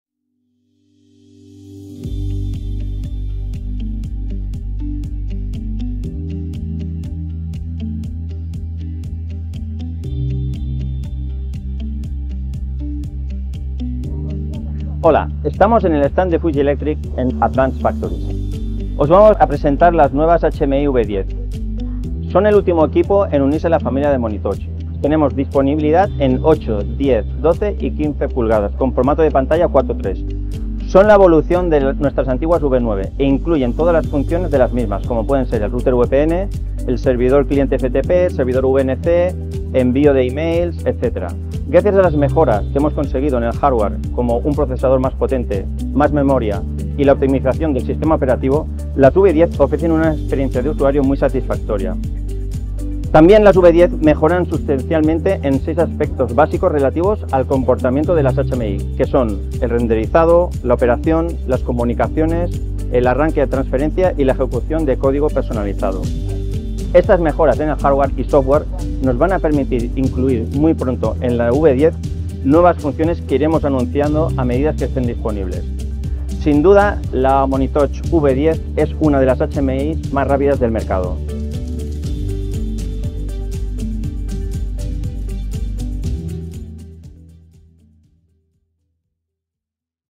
(Spanish) Monitouch V10 en Advanced Factories
La Monitouch V10 es la HMI más rápida del mercado. Conoce más detalles sobre nuestra HMI.